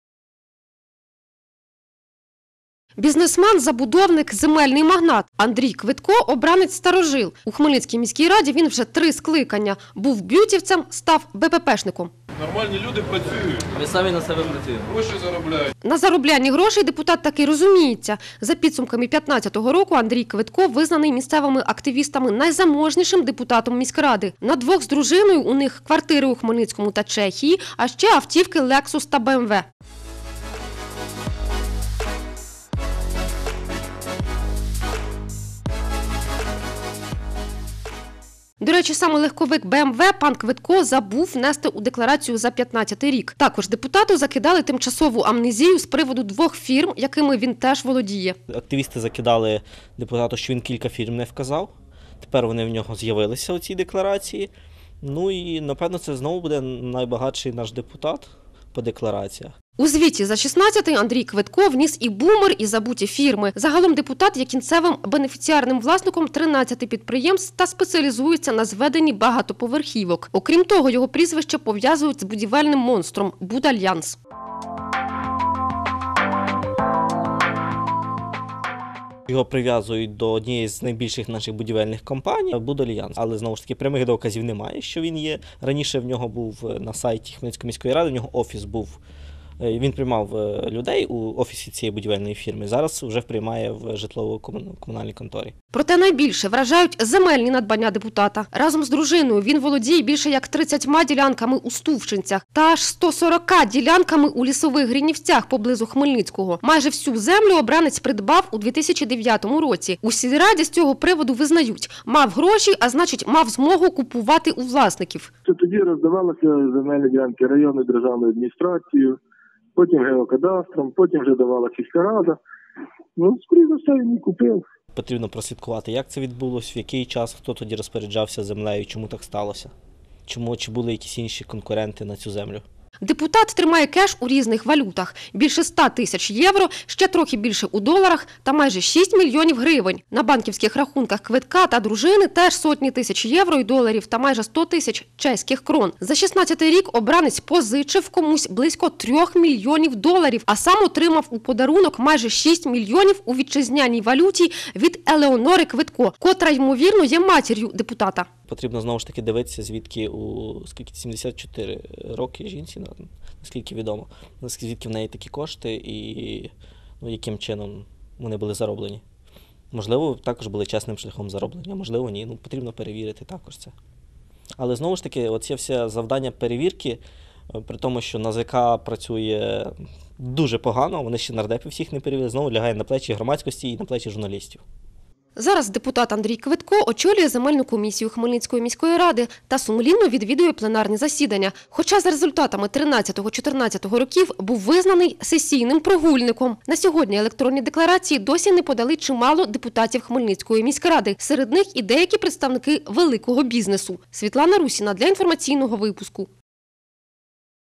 Хто серед хмельницьких депутатів Рокфеллер
Хто у раді всіх багатше? Наші журналісти дослідили декларації хмельницьких депутатів та побачили, хто може претендувати на звання місцевого Рокфеллера. До слова, про свої статки встигли прозвітувати лише деякі обранці Хмельницької міської ради. Утім, вже зараз можна передбачити: найзаможніший – це Андрій Кветко.